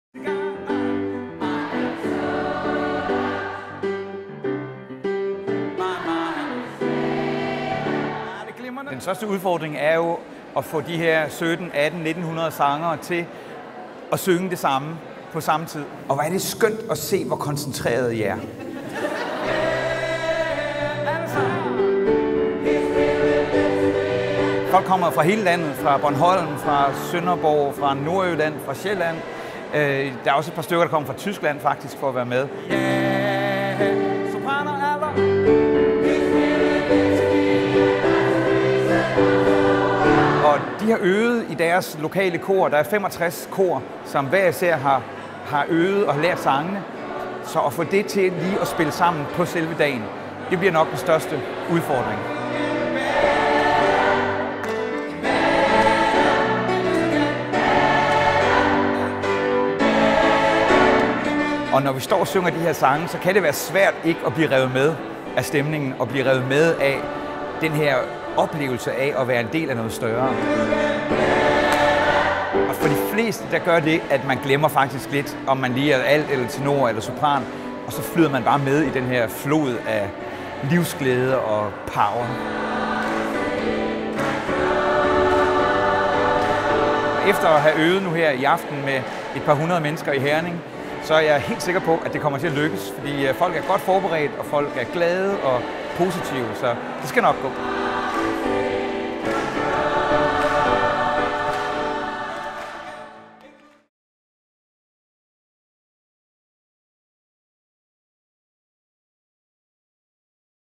Over 1700 i stort gospelkor til Det Himmelske Show
Der har været fælles korøvelser i København, Århus og Herning inden de over 1700 sangere samles til Det Himmelske Show i Jyske Bank Boxen den 1. juni. Køb billet til showet her